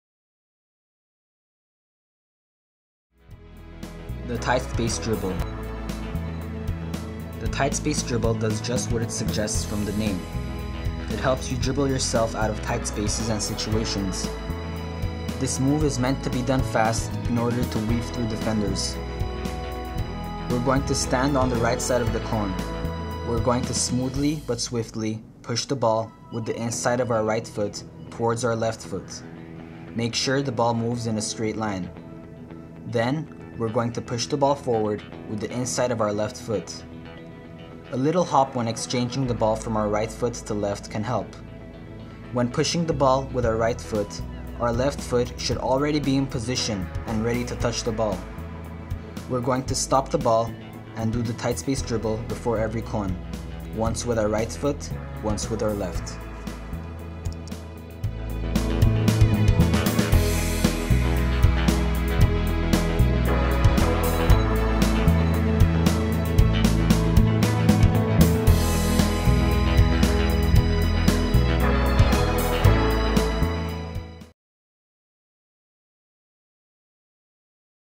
Symmetrical Soccer - Tight-space-dribble
Scene from the Symmetrical Soccer training video.
Breakdown of a training exercise... Symmetrical Soccer - Tight-space-dribble.